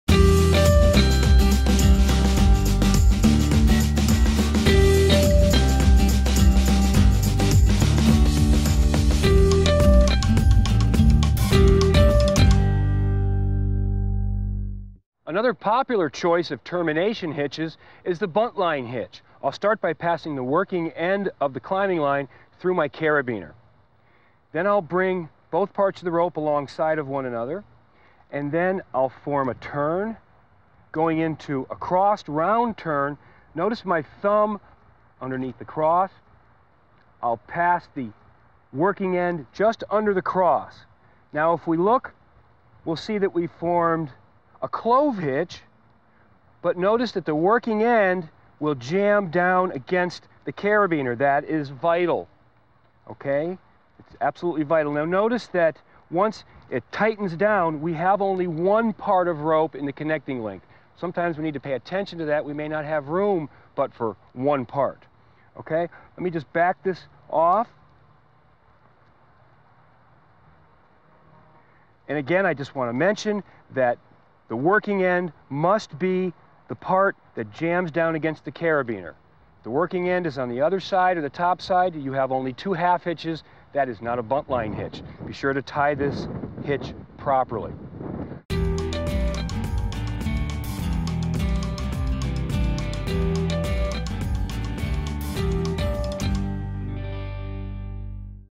The Buntline Hitch
A buntline hitch can be incorporated as an anchor hitch in most connecting links. It is extremely useful when attaching cordage to connecting links with limited available space, such as rope snaps or captive-eye carabiners. This video explains why it is crucial that the working end of the cordage is positioned correctly.